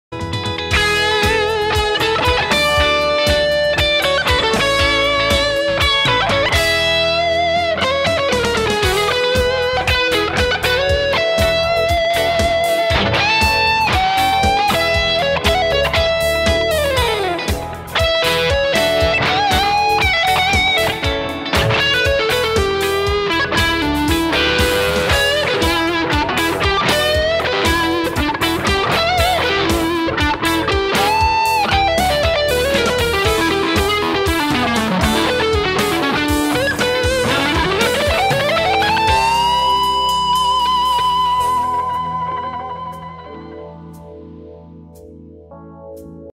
Deniz Alatas - Guitar Solo for the next BOOM a LOON Song
This is the guitar solo for the next BOOM a LOON tune. Coming up next Sunday!

Gear:
Nik Huber Guitars Orca59
Bluguitar Amp 1
Blackstar LT DIST.
XOTIC RC BOOSTER
Tc Electronic Flashback Mini Delay